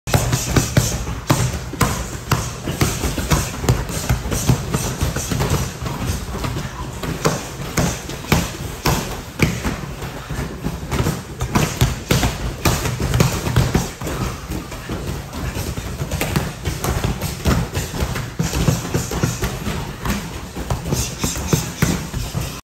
Ground and pound
Working on our ground striking with our cage fitness bags.
Train hard and often – Osu!

Kancho says: Train hard and often – Osu!